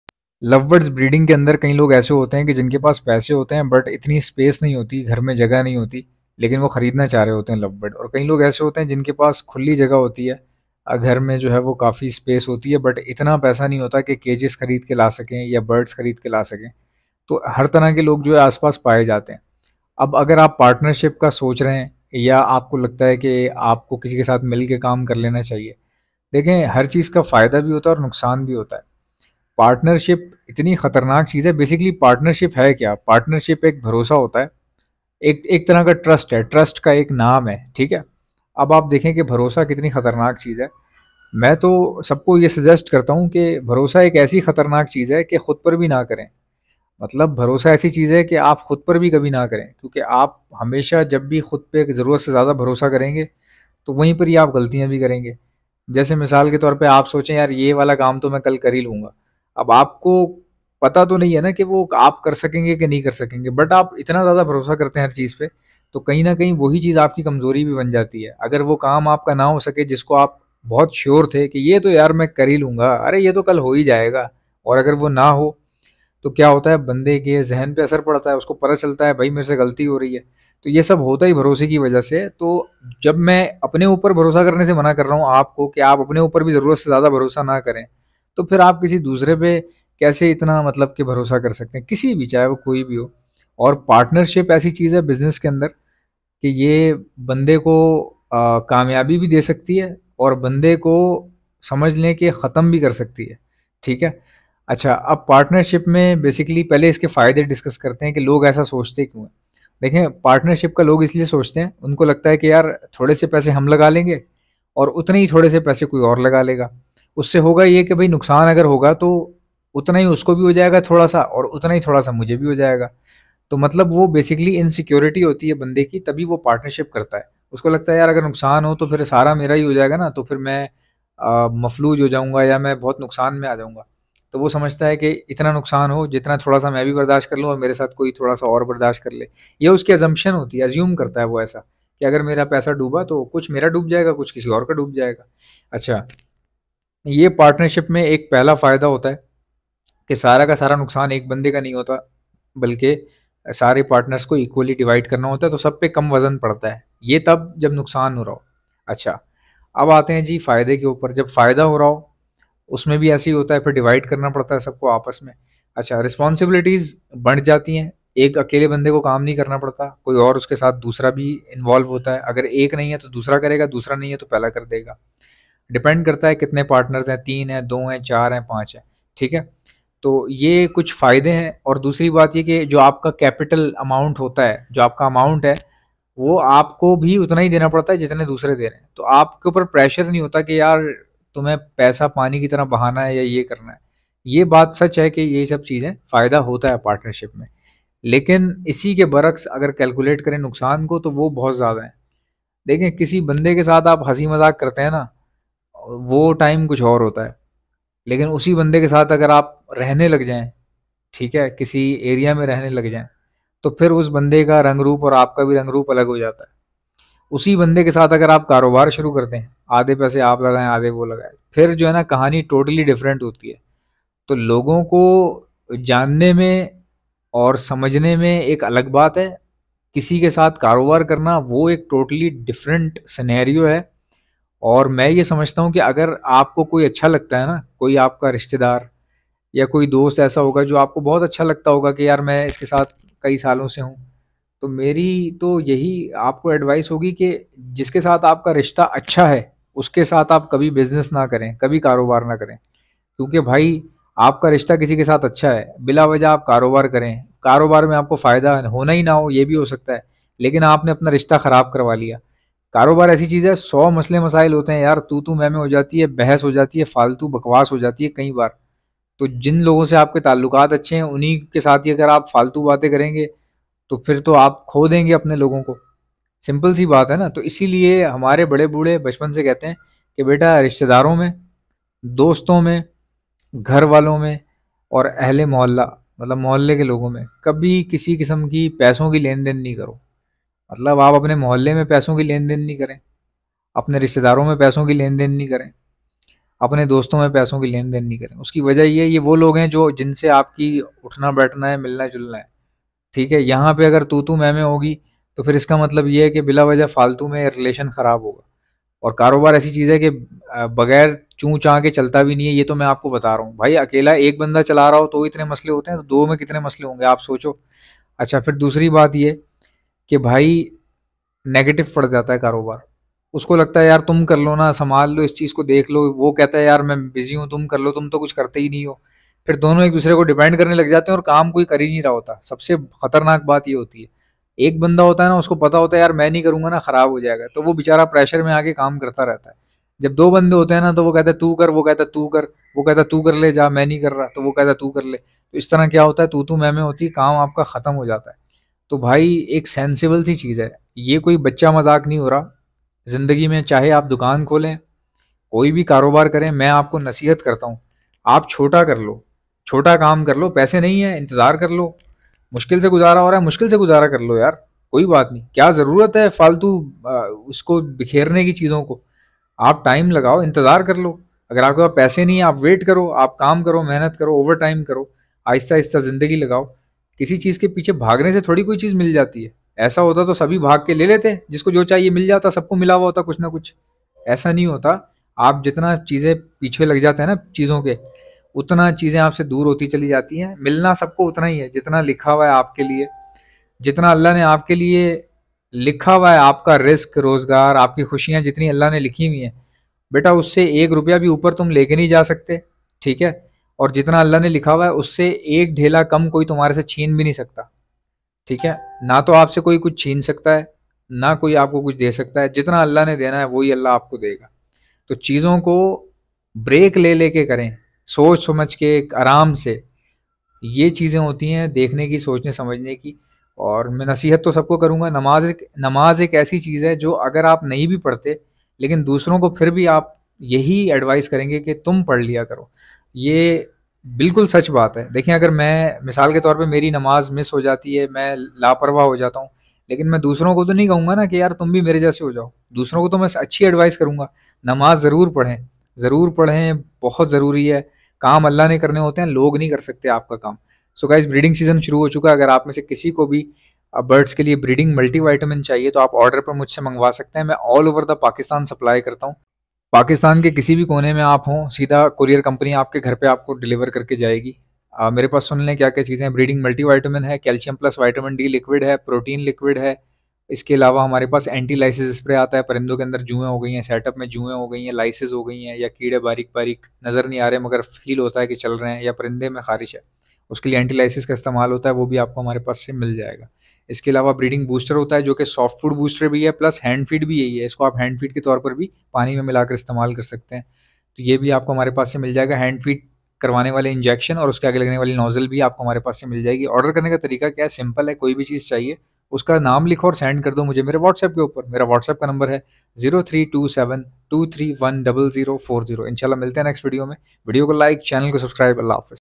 Partnership krne k bad ka Haal
lovebirds breeding me partnership krne k bad ka Haal

results after making partnerships in lovebirds breeding business